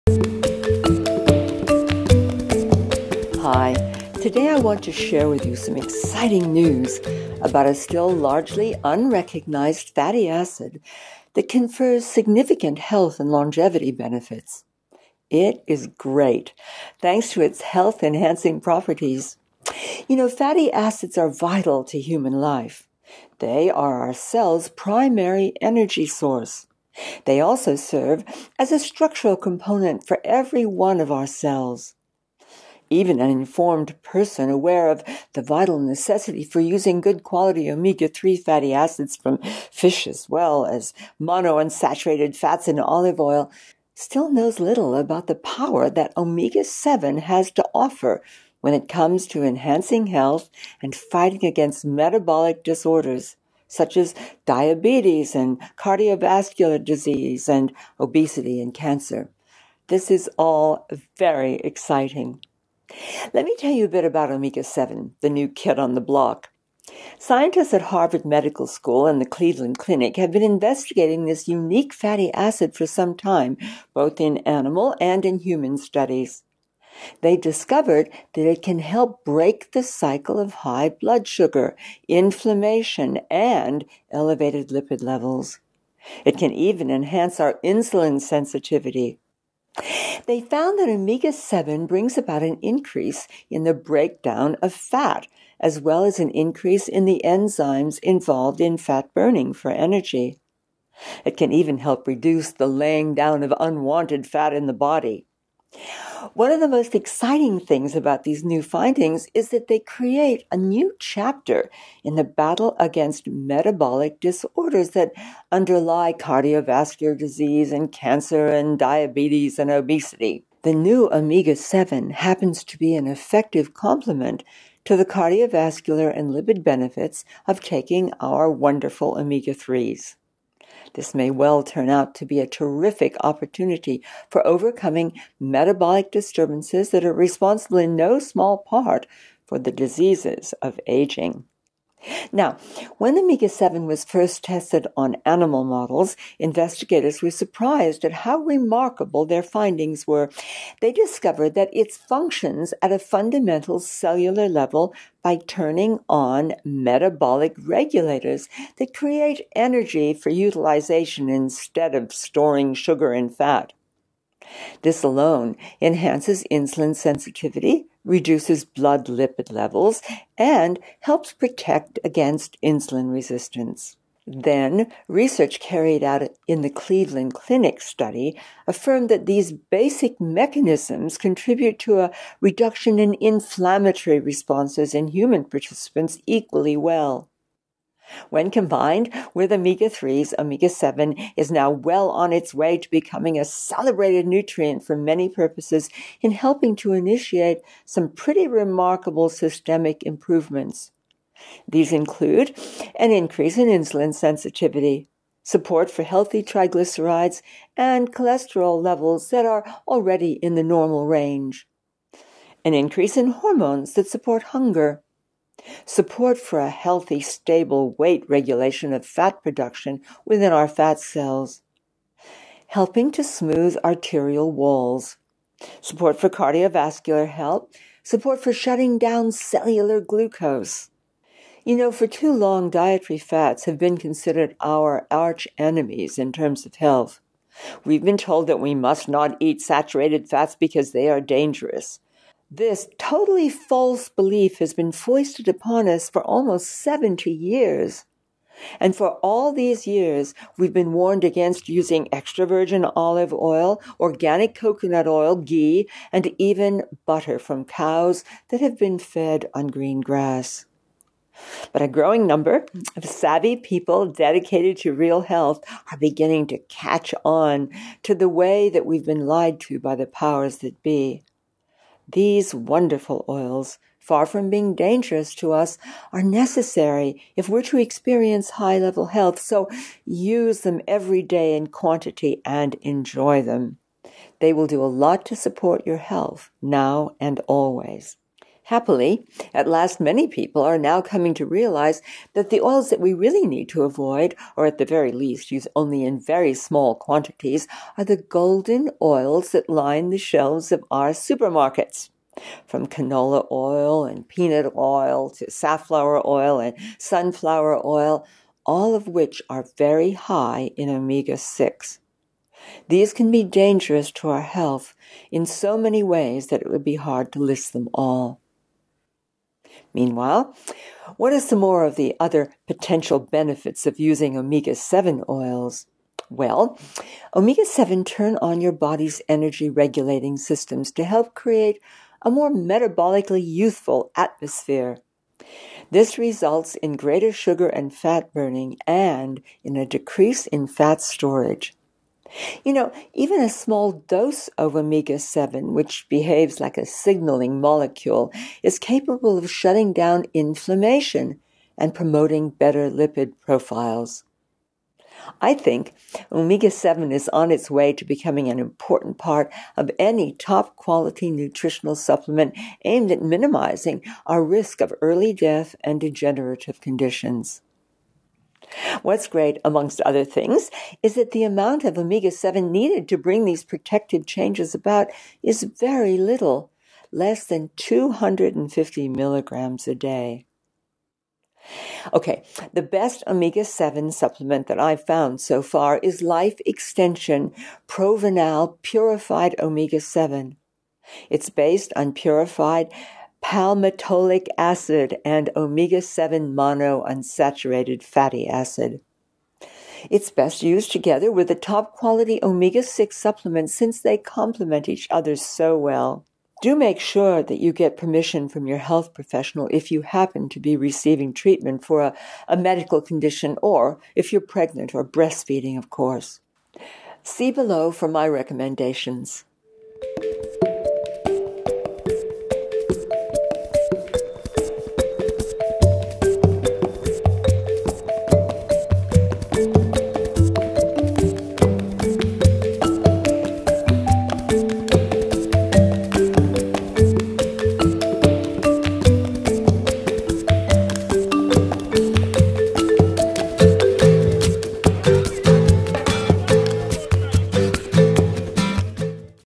Sacred Truth Ep. 53: Fabulous New Omega-7
I want to share with you some exciting news about a still largely unrecognized fatty acid that confers significant health and longevity benefits. It’s great, thanks to its health enhancing properties. Fatty acids are vital to human life. They are our cells’ primary energy source. They also serve as a structural component for every one of our cells.

They’ve found that Omega-7 brings about an increase in the breakdown of fat as well as an increase in the enzymes involved in fat burning for energy. It can even help reduce the laying down of unwanted fat in the body. One of the most exciting things about these findings is that they create a new chapter in the battle against metabolic disorders that underlie cardiovascular disease, obesity, cancer, and diabetes. The new Omega-7 happens to be an effective complement to the cardiovascular and lipid benefits of taking Omega-3s. This may well turn out to be a terrific opportunity for overcoming metabolic disturbances responsible, in no small part, for the diseases of aging.

When Omega-7 was first tested on animal models, investigators were surprised at how remarkable the findings were. They discovered that it functions at fundamental cellular levels by turning on metabolic regulators that create energy for utilization instead of storing sugar and fat. This alone enhances insulin sensitivity, reduces blood lipid levels, and helps protect against insulin resistance. Research carried out in the Cleveland clinic study affirmed that these basic mechanisms contribute to a reduction in inflammatory responses in human participants equally well.

When combined with Omega-3s, Omega-7 is now well on its way to becoming a celebrated nutrient for many purposes in helping to initiate some remarkable systemic improvements. These include:

an increase in insulin sensitivity
support for healthy triglycerides and cholesterol levels already in normal range
an increase in hormones that counter hunger
support for a healthy stable weight
regulation of fat production within fat cells
helping to smooth arterial walls
support for cardiovascular health
support for shutting down cellular glucose.

For too long dietary fats have been considered our arch-enemies in terms of health. We've been told we must not eat saturated fats because they are dangerous. This false belief has been foisted upon us for almost 70 years. For so many years we have been warned against using extra-virgin olive oil, organic coconut oil, ghee, and even butter from cows that have been fed on green grass.

A growing number of savvy people dedicated to real health are beginning to catch on to the way we have been lied to by the powers that be. These wonderful oils, far from being dangerous to us, are necessary if we are to experience high-level health. Use them every day in quantity and enjoy.  The do a lot to support your health now and always

Happily, at last, many people now are coming to realize that the oils that we need to avoid, or at the very least use only in very small amounts, are the golden oils that line the shelves of our supermarkets—from canola oil and peanut oil to safflower oil and sunflower, all of which are very high in Omega-6. These can be dangerous to health in so many ways that it would be hard to list them all.

Meanwhile, what are some of the other potential benefits of using Omega-7 oils?

I. Omega-7 turns on your body’s energy regulating systems to help create a more metabolically youthful atmosphere. This results in greater sugar and fat burning, and a decrease in fat storage.
II. Even a small dose of Omega-7 behaves like a signaling molecule, which is capable of shutting down inflammation and promoting better lipid profiles.
III. Omega-7 is on its way to becoming an important part of any top quality nutritional supplement aimed at minimizing the risk of early death and degenerative conditions.

What's great amongst other things is that the amount of Omega-7 needed to bring these protective changes about is very little —less than 250 mg per day.

The best Omega-7 supplement I’ve found is Life Extension Provinal Purified Omega-7. It's based on purified Palmitoleic acid and Omega-7 monounsaturated fatty acid.

It’s best used together with a top quality Omega-6 supplement since they complement each other so well. Do make sure you get permission from your health professional if you happen to be receiving treatment for a medical condition, or are pregnant or breastfeeding of course.

It is also available from amazon